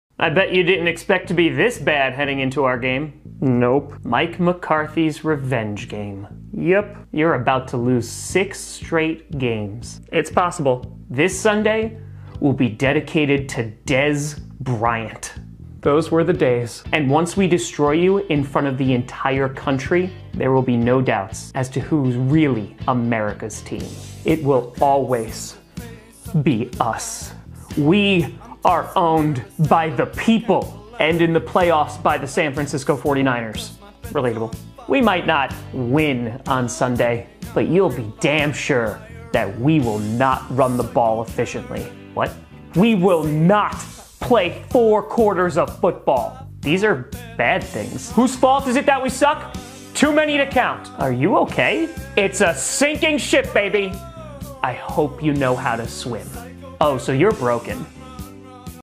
Packers Fans vs Cowboys Fans (NFL Week 10)
The Green Bay Packers take on the Dallas Cowboys in Week 10 of the NFL.